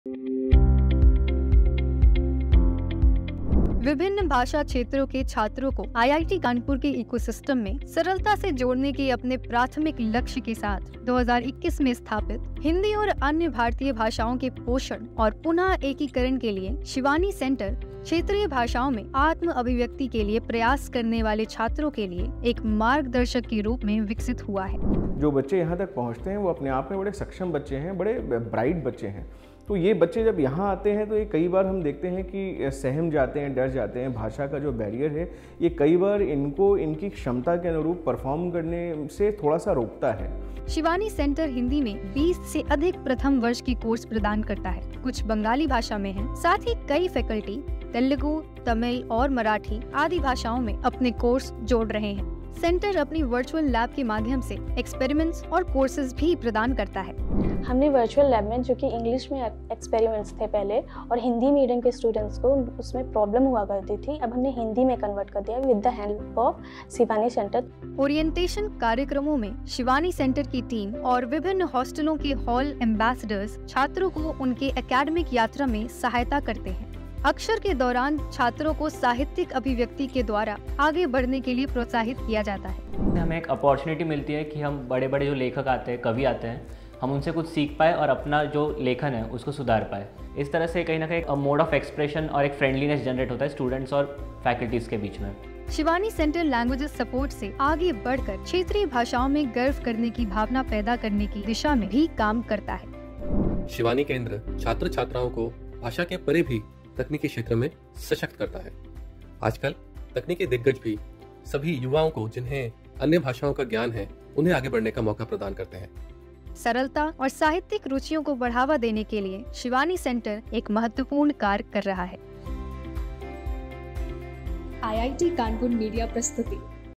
World Hindi Day I Shivani Center I IIT Kanpur
The Shivani Centre for the Nurture and Re-integration of Hindi and Other Indian Languages at IIT Kanpur has become not just a salient space for providing a soft landing and easing students from Hindi and other language backgrounds into the institute's academic ecosystem, but has also taken shape of promoting literary culture and a sense of pride in our language.

Here is a look at its incredible journey and impact.